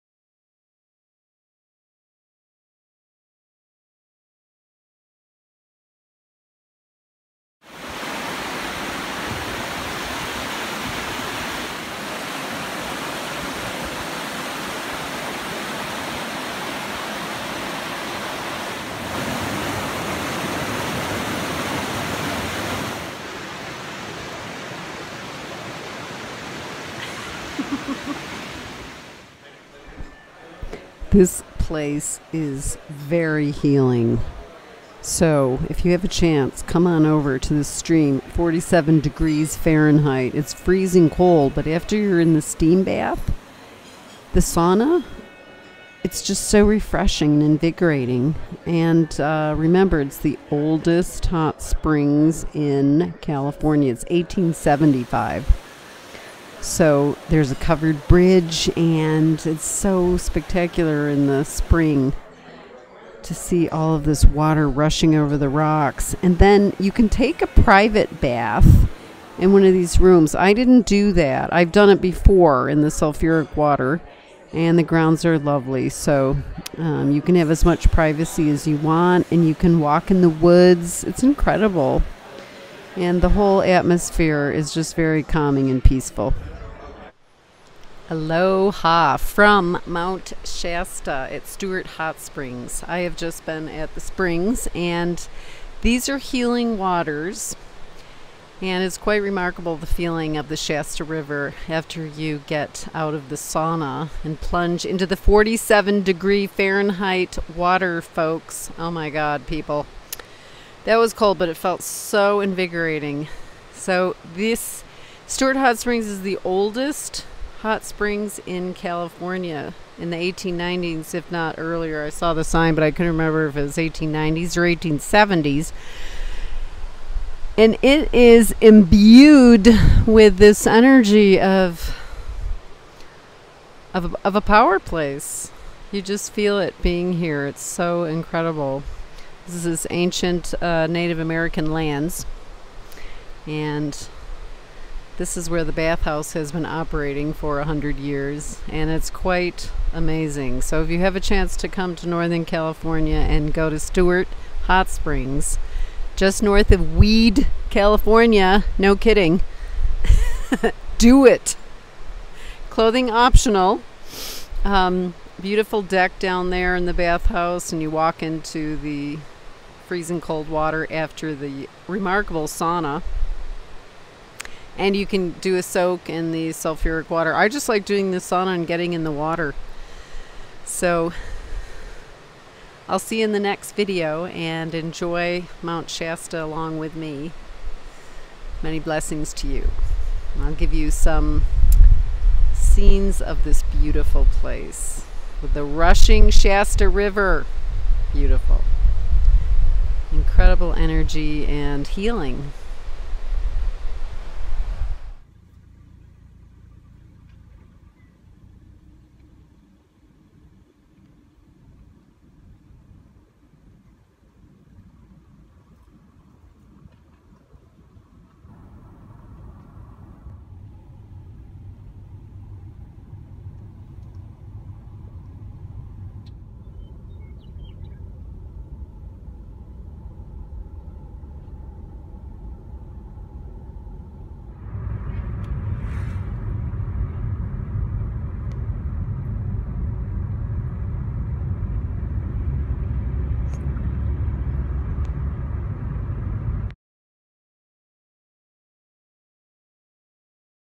Mount Shasta, California, Stewart Mineral Springs
Mount Shasta Stewart Hot Springs

Mount Shasta shines with so many amazing and wow worthy offerings. I miss it all ready. There is a sacred vibe to this magical mountain and it penetrates your soul and mind. I could barely tear myself away.

Stewart Hot Springs is rejuvenating -- it really recharges your batteries.

Let me know if you want to learn yoga and photography in a luxury retreat here in the
mountains.

The Author’s 6 Books on Amazon